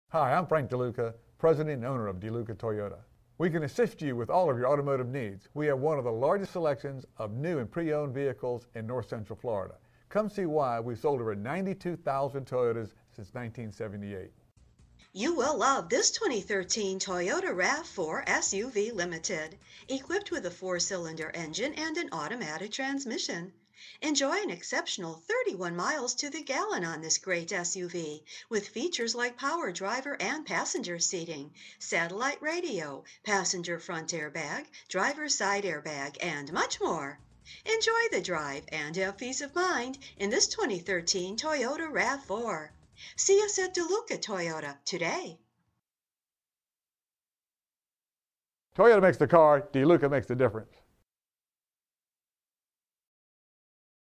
2013 Toyota RAV4 Deluca Toyota Ocala, FL 34474
Toyota RAV Check out this Classic Silver Metallic Toyota Rav4 SUV, equipped with a 4 Cylinder engine and an automatic transmission. Enjoy an exceptional 31 miles to the gallon on this great SUV with features like Power Driver and Passenger Seating, Satellite Radio, Tinted Glass, Passenger's Front Airbag, Driver Side Air Bag, Leather Upholstery, Power Sunroof, Anti-Lock Braking System, Bluetooth System, Alloy Wheels, Heated Seat, Keyless Entry, 12 Volt Power Source, Back Up Camera, Navigation System, and much more. Enjoy the drive and have peace of mind in this Toyota Rav4. See us at Deluca Toyota today!
MILES:  1,896
VIN:2T3YFREV8DW044338

Deluca Toyota
1719 S.w.College Rd.